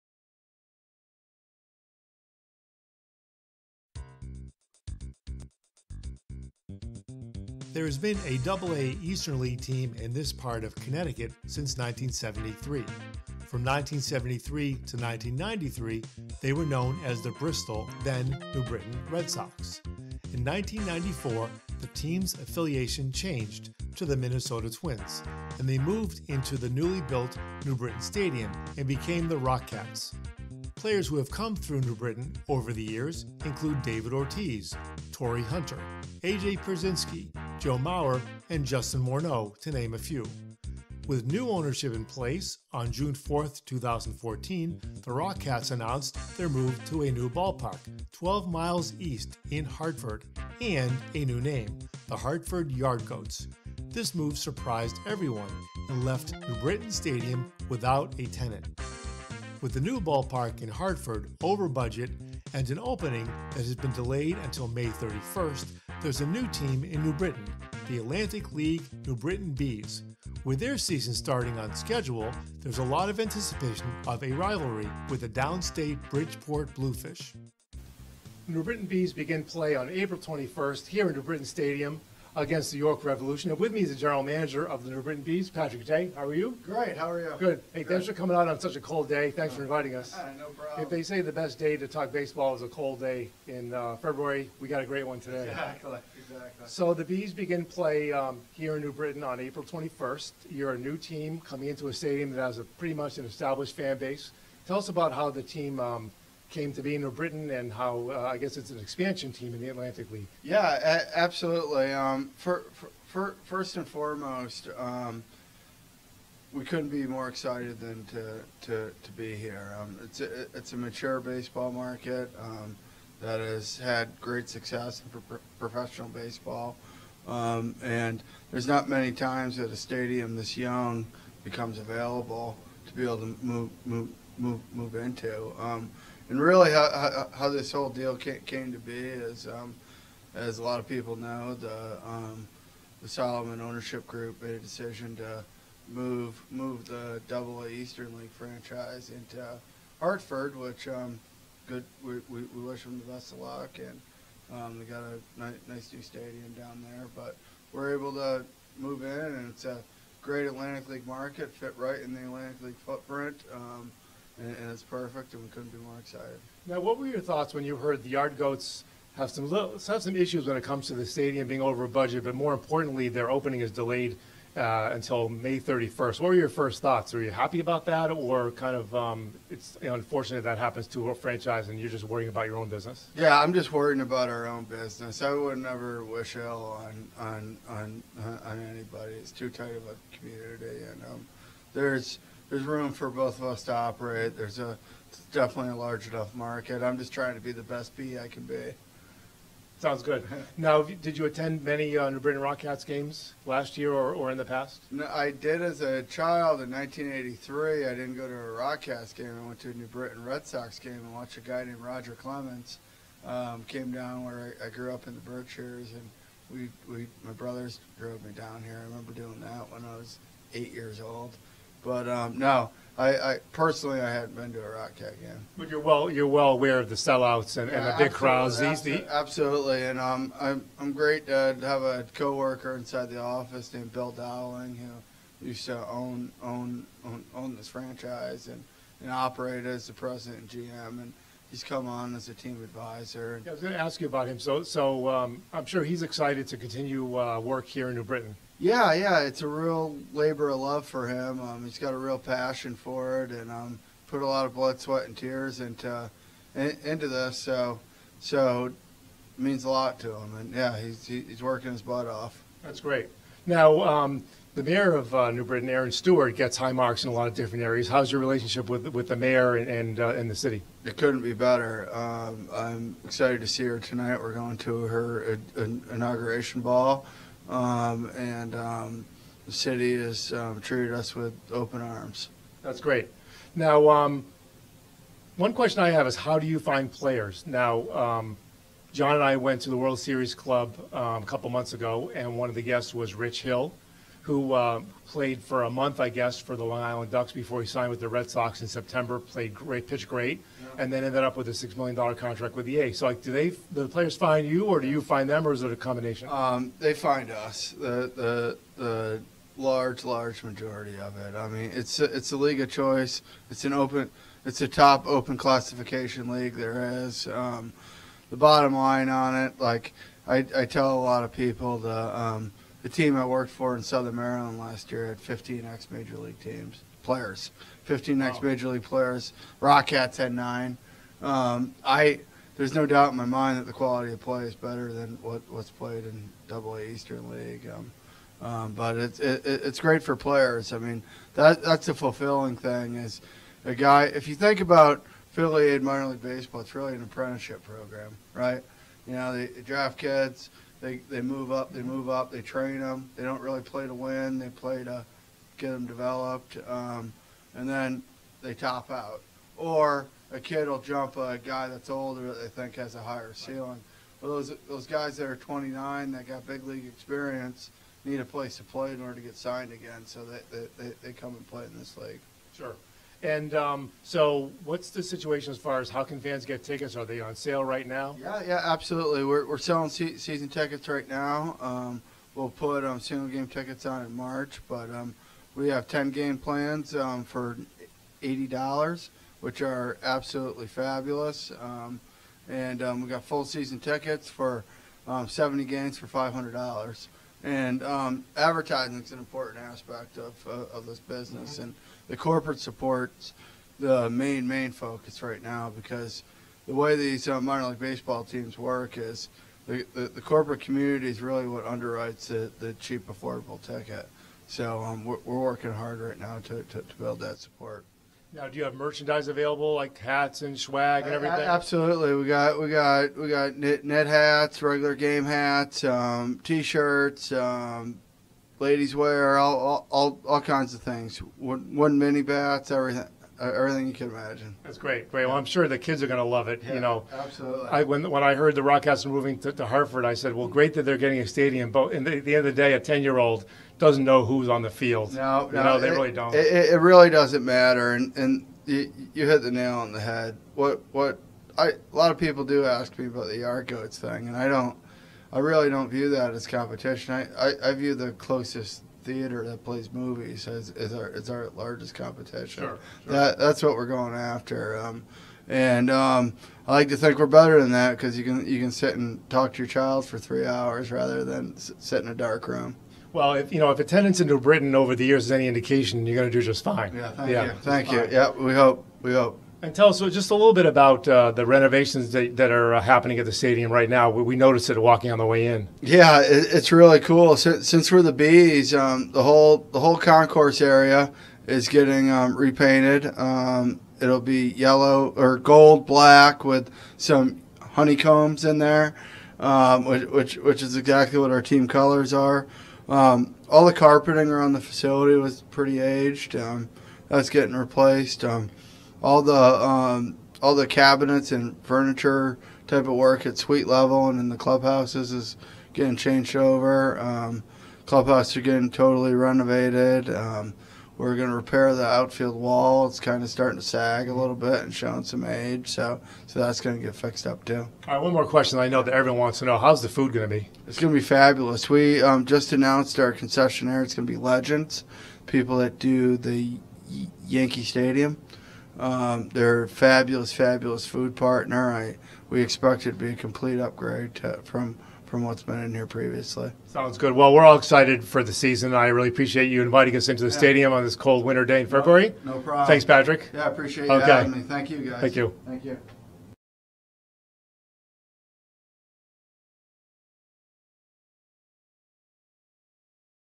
Your Boston Sports - New Britain Bees GM Interview
Your Boston Sports visited with the GM of the Atlantic League New Britain (CT) Bees, Patrick Day